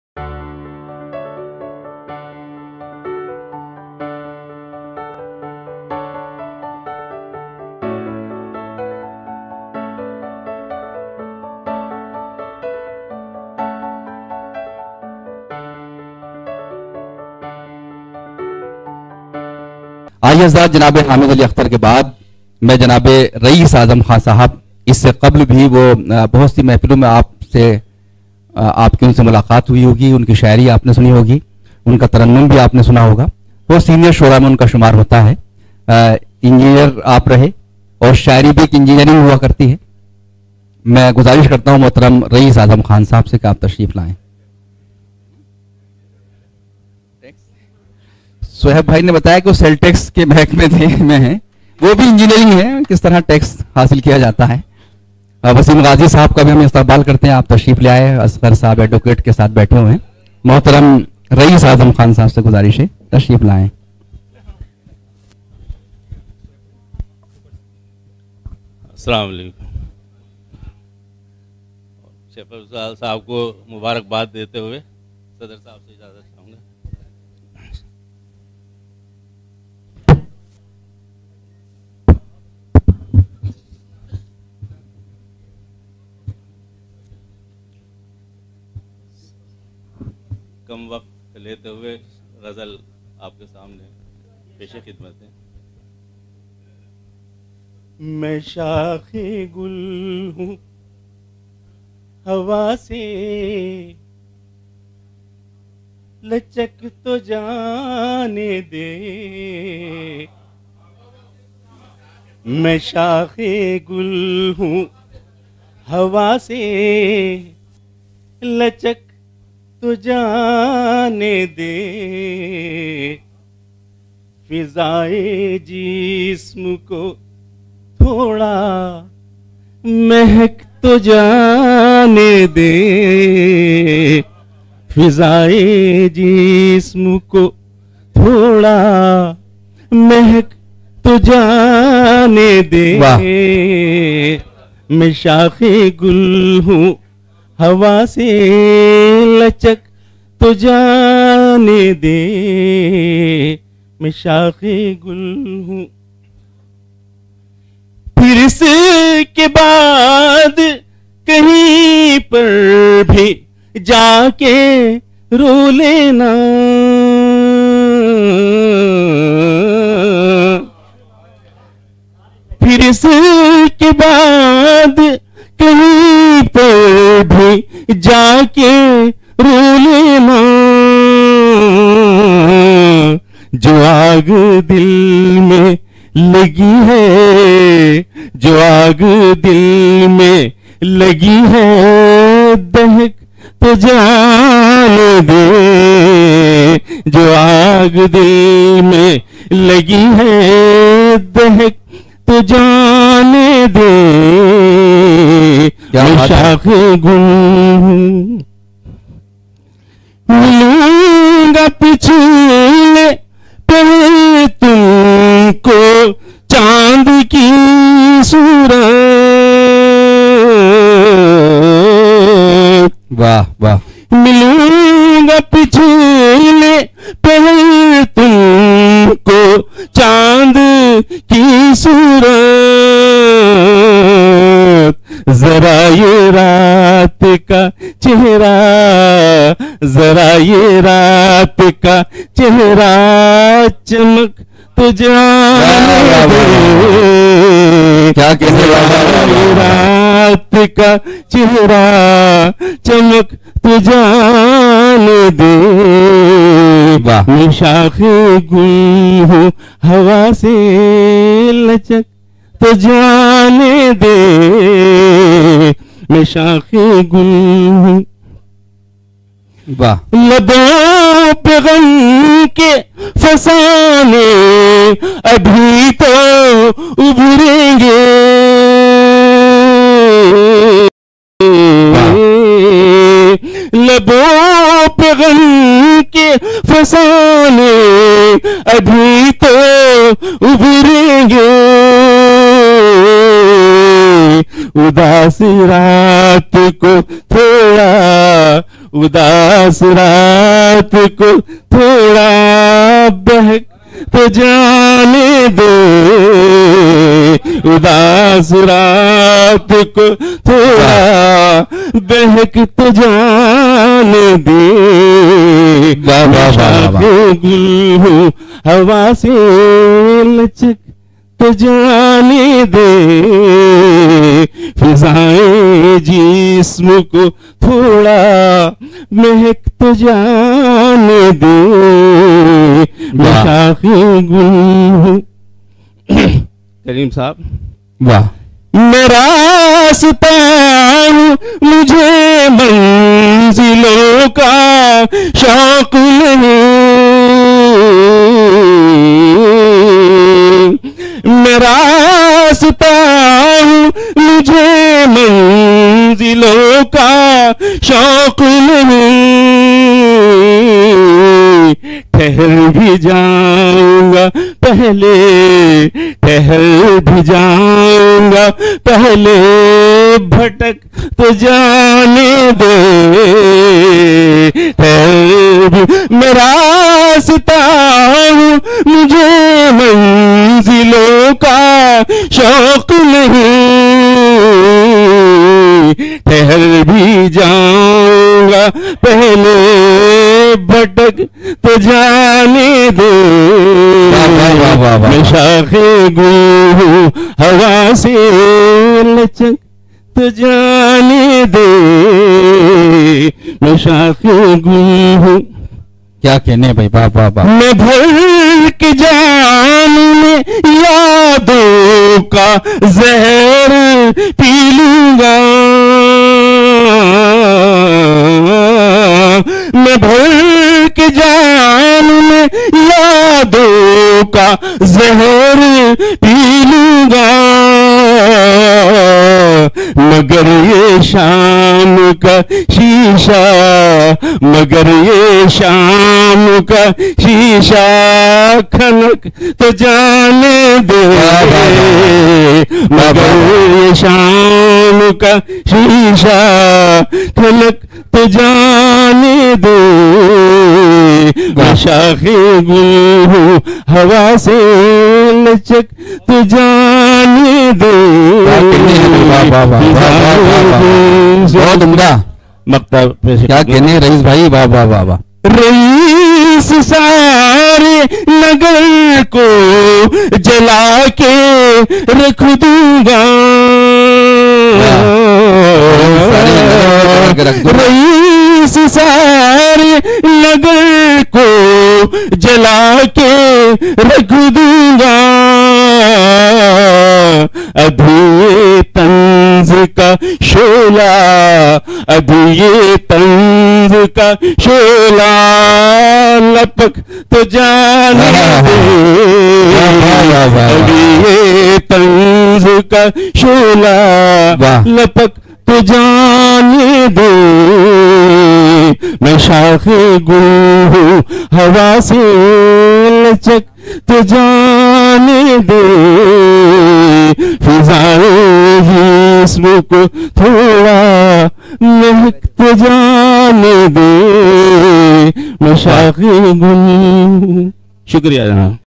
RAEES AZAM KHAN,ALL INDIA MUSHAIRA,HOTEL RIVER VIEW,OKHLA,ON 13TH DEC 2017,ORG-KALEEMUL HAFEEZ
RAEES AZAM KHAN,
HOTEL RIVER VIEW PRESENTS
FELICITATION AND POETIC SYMPOSIUM,
ORGANISED BY KALEEMUL HAFEEZ(HILAL MALIK)
CHAIRMAIN-AL HAFEEZ EDUCATIONAL ACADEMY
CMD-HOTEL RIVER VIEW,OKHLA,NEW DELHI.
CONVINOR-JB.SUHAIB FAROOQUI
ON 13TH DEC 2017.
PRESIDE BY-MR.MAJEED MEMON(HON'BLE MP)
FELICITATING-PROF.SHAHPAR RASOOL(VICE CHAIRMAN-DELHI URDU ACADEMY)